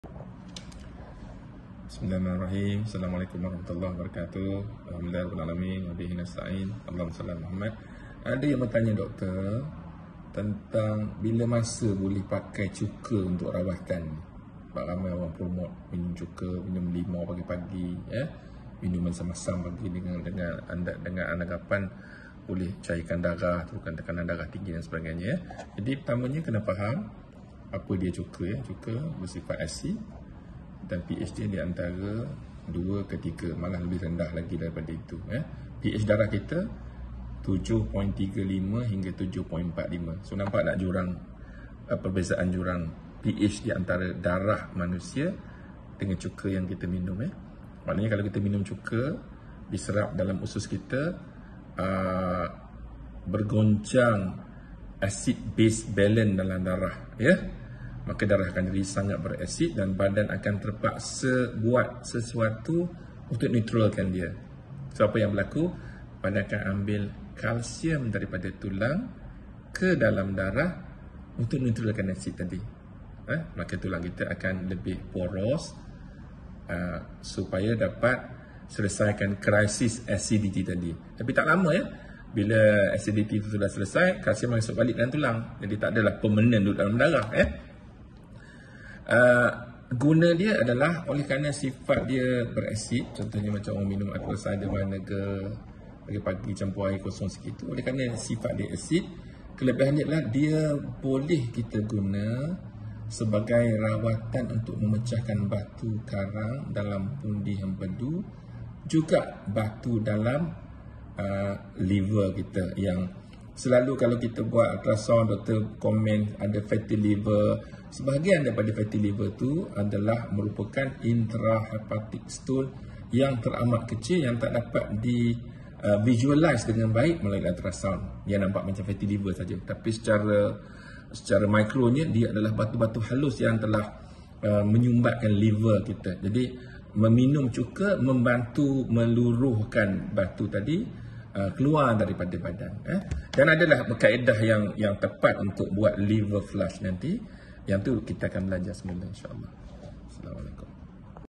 Pro dan Cons Cuka
Apakah kelebihan dan kekurangan cuka dalam rawatan? Dengarkan penjelasan Dr Razin Jaafar.